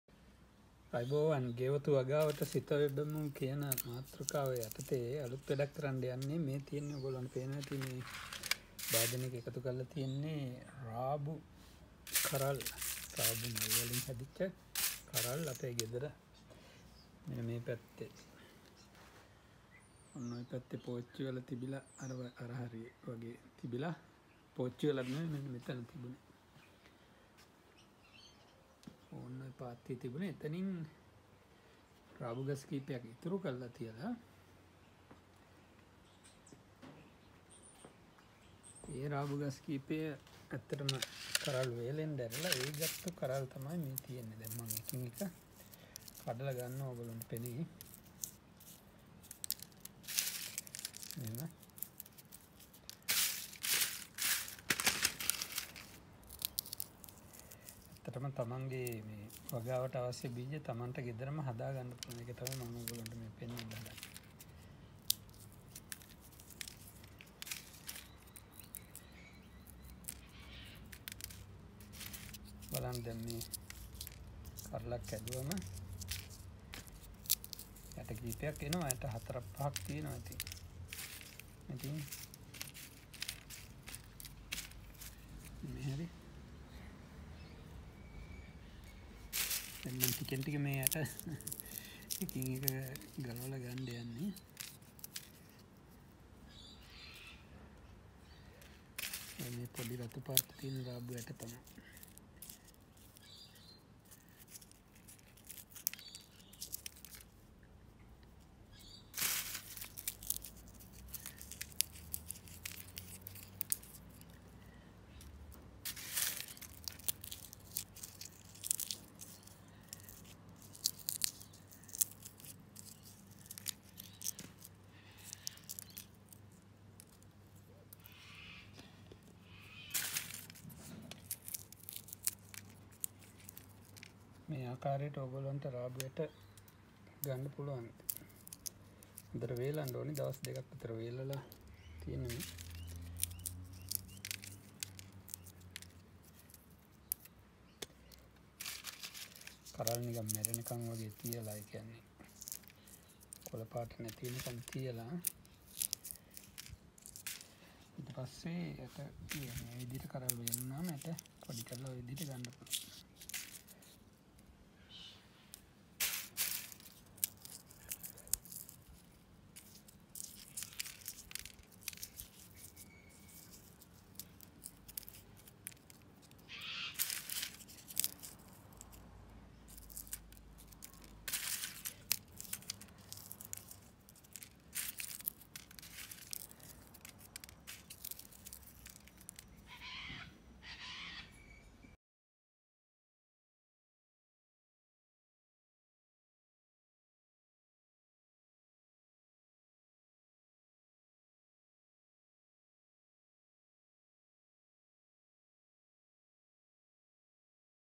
ගෙවත්තේ රාබු වගාවෙන් ඊලඟ කන්නයට බීජ සකස් කර ගනිමු -ගෙවතු වගාවට සිත යොදමු 3 gardening tips 3
@KeerthiKulasekara අද ඔයලට කියලා දෙන්නේ  ගෙවත්තේ රාබු වගාවෙන්  ඊලඟ කන්නයට බීජ සකස් කර ගන්න හැටි.. ඔබේ රාබු වගවෙන් පැළ කිහිපයක් මල් සහ කරල් හටගන්නට තියලා ඊට පස්සේ එන කරල් වේලලා මේ විදිහට  ඊලඟ කන්නයට අවශ්‍ය ඇට ( බීජ ) ලබාගන්න පුළුවන්